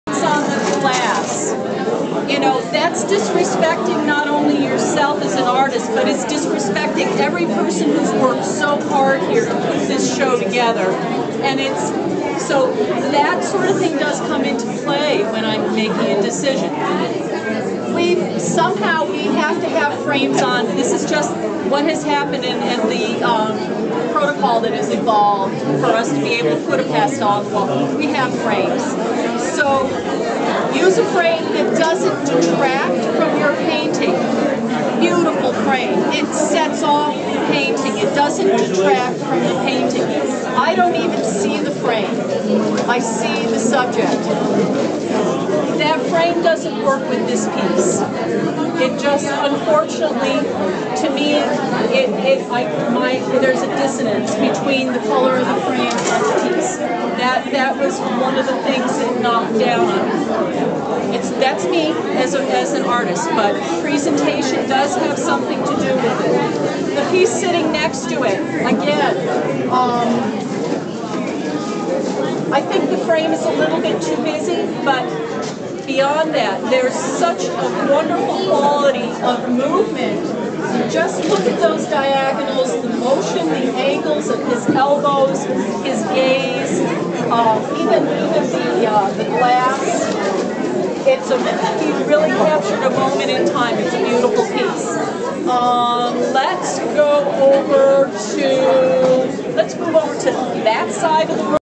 #2 Liz Hayward-Sullivan review of SPS 16th Annual Show
Southeastern Pastel Society 16th Annual International Exhibit
Liz Haywood Sullivan reviewing the show
part 2